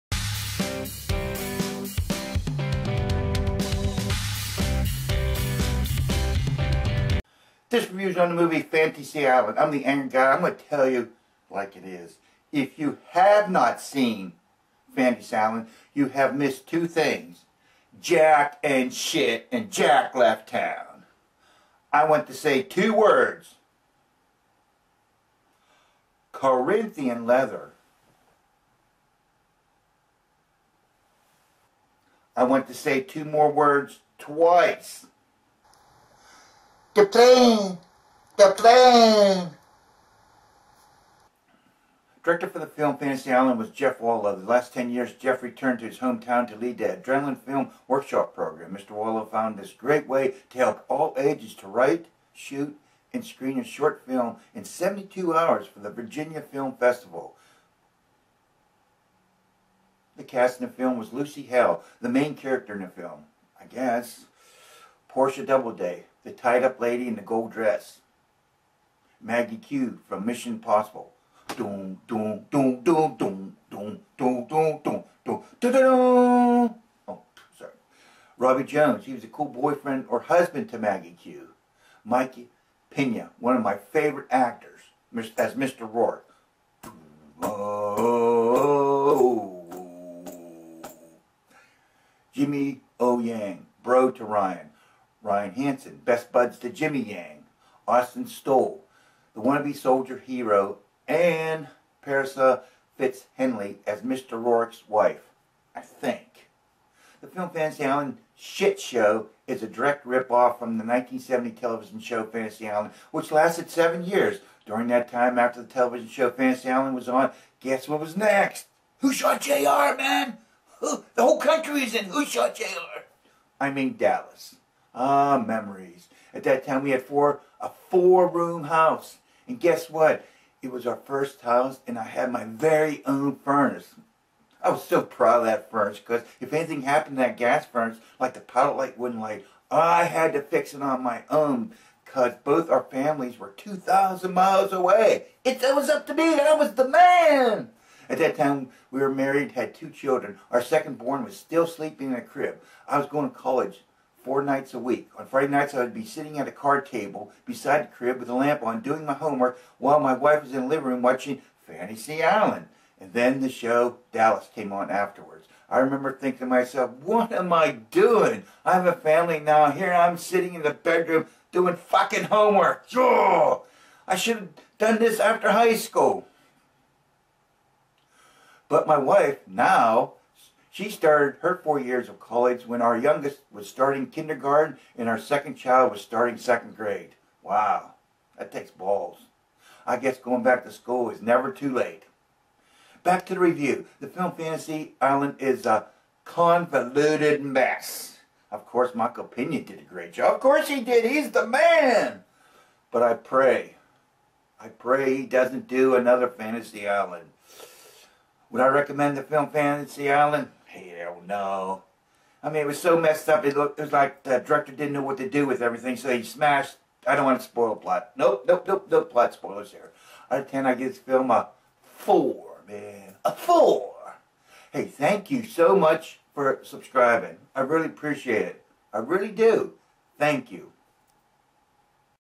Fantasy Island Movie Review 2020
The movie Fantasy Island is a terrible rip off from the 1970's television show that lasted 7 years.